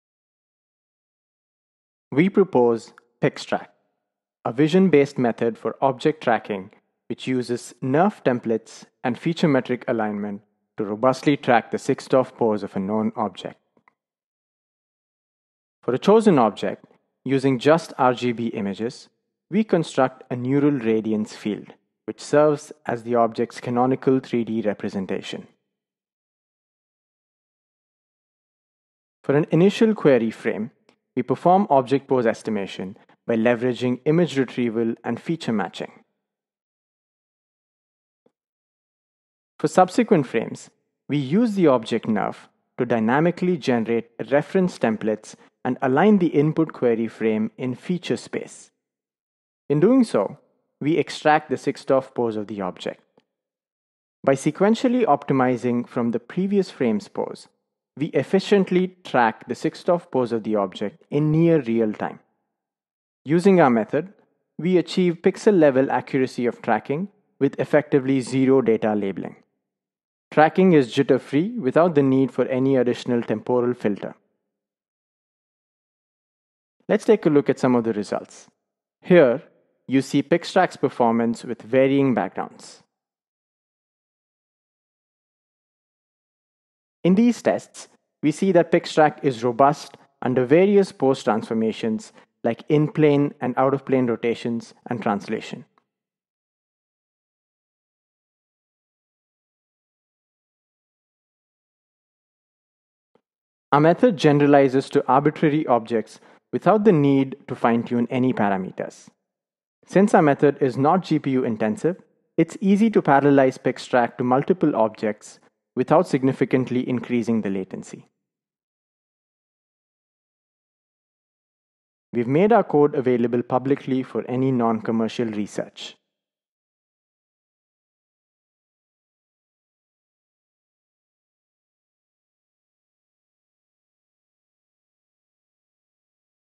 PixTrack - from Giant AI
We present PixTrack, a vision based object pose tracking framework using novel view synthesis and deep feature metric alignment. Our evaluations demonstrate that our method produces highly accurate, robust, and jitter-free 6DoF pose estimates of objects in RGB images without the need of any data annotation or trajectory smoothing. Our method is also computationally efficient making it easy to have multi-object tracking with no alteration to our method and just using CPU multiprocessing.